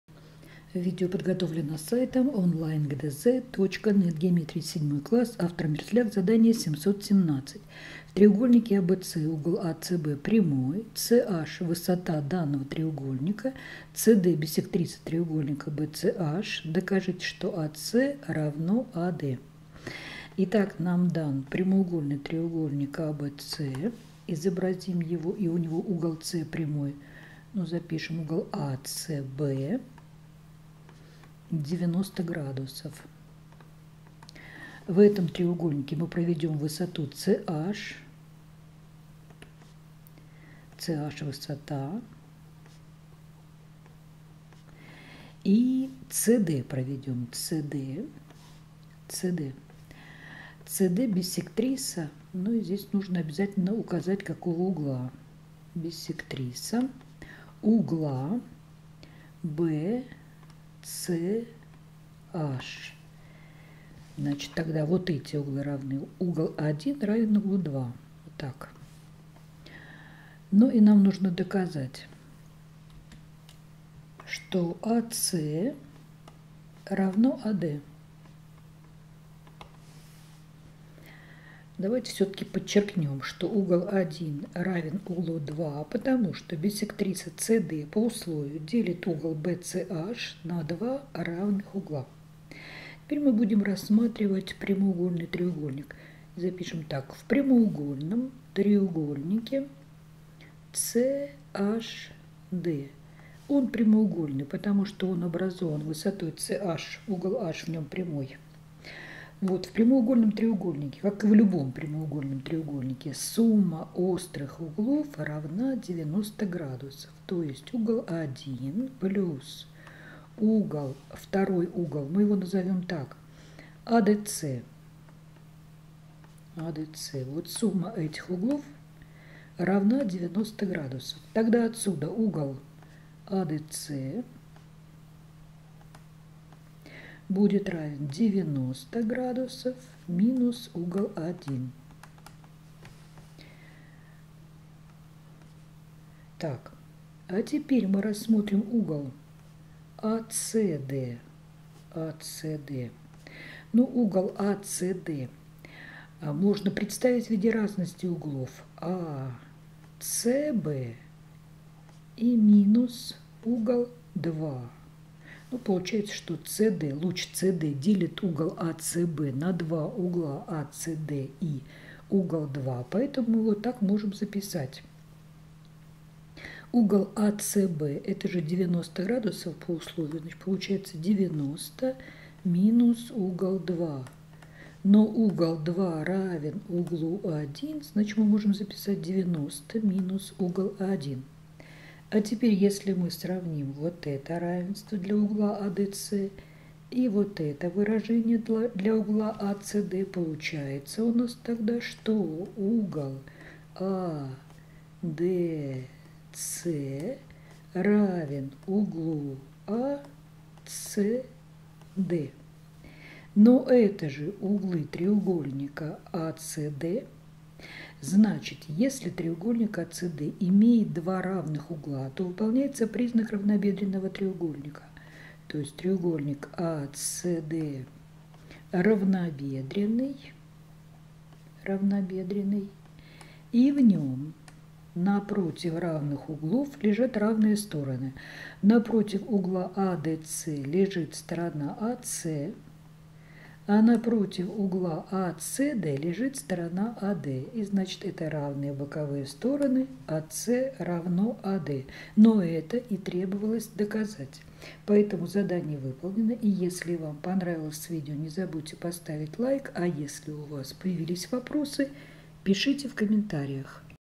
Задание №717 - ГДЗ по геометрии 7 класс (Мерзляк)
Доказательство по условию задания 717 необходимо построить, используя равенство 90 градусам суммы острых углов прямоугольного треугольника. Также удобно воспользоваться свойством внешнего угла треугольника, - он равен сумме двух углов треугольника, не смежных с ним.